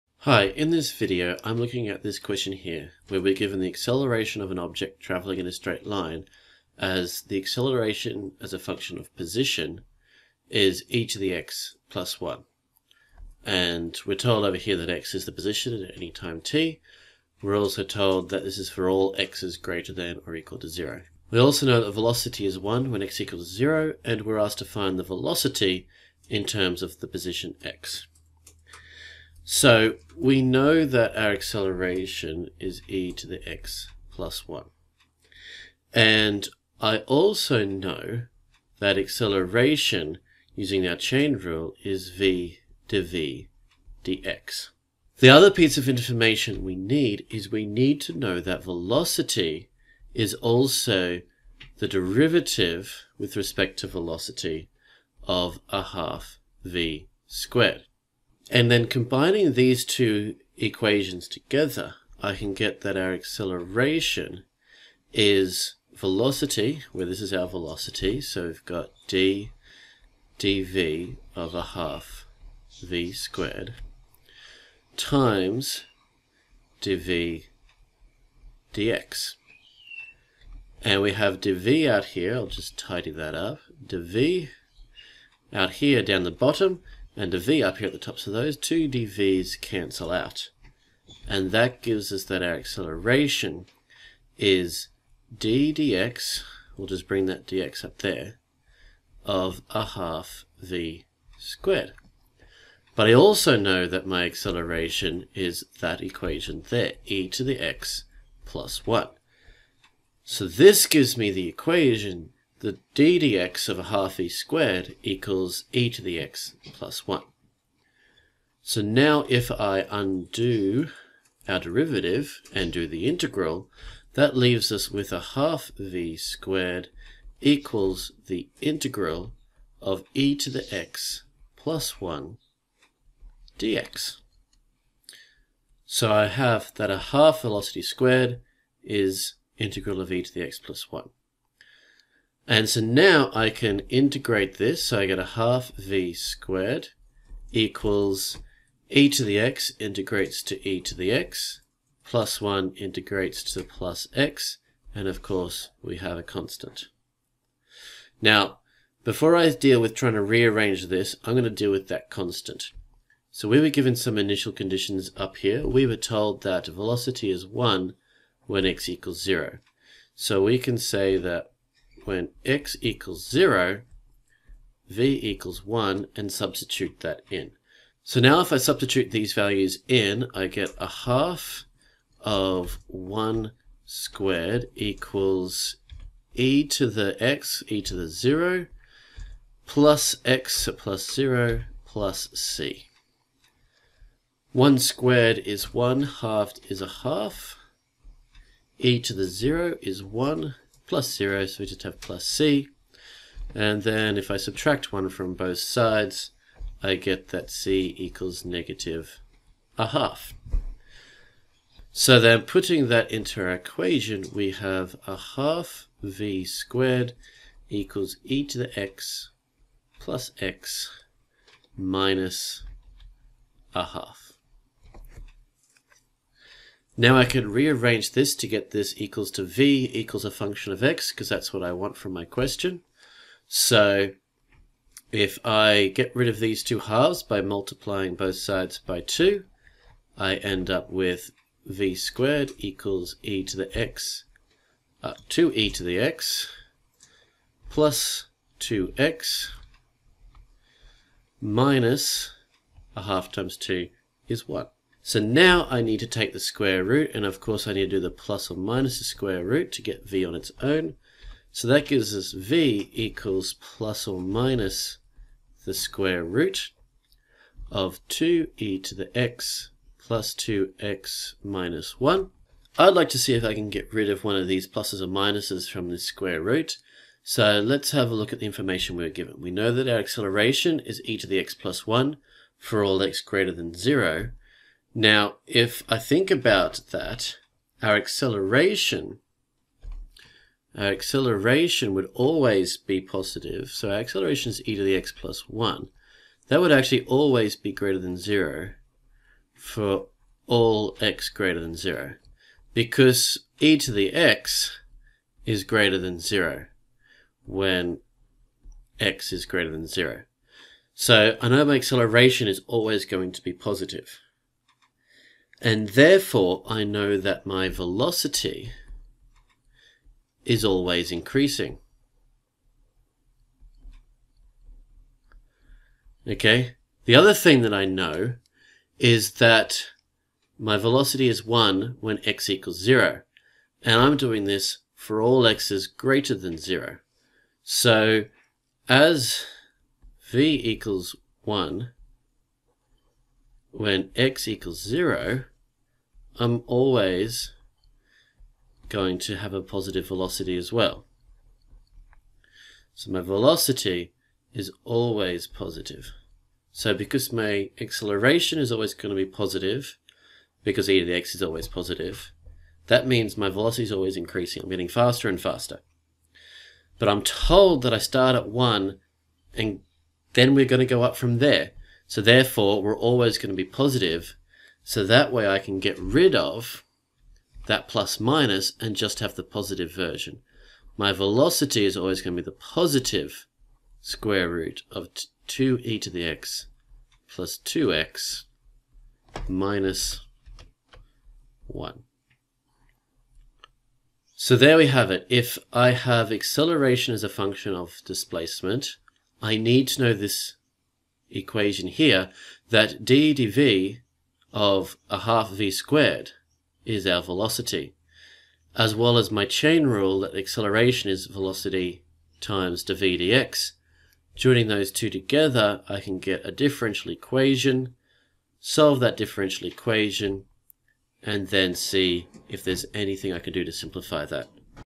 Acceleration as a Function of Position
This video demonstrates with an example how to determine the velocity as a function of position if given the acceleration as a function of position. This uses differential equations, and calculus